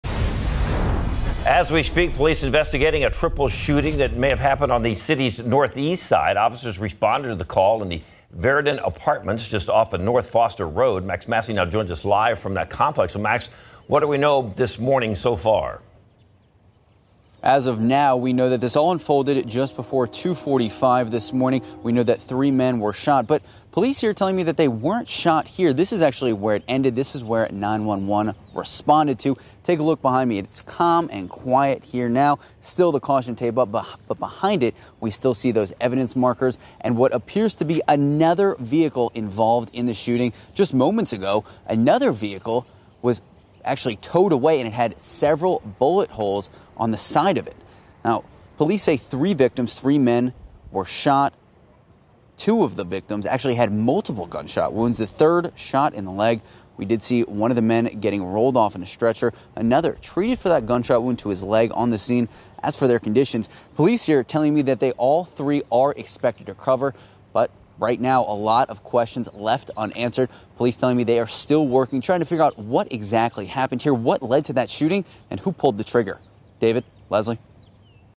3 hospitalized in shooting on NE Side, police say
San Antonio police are investigating after three people were shot on the city’s Northeast Side early Monday morning.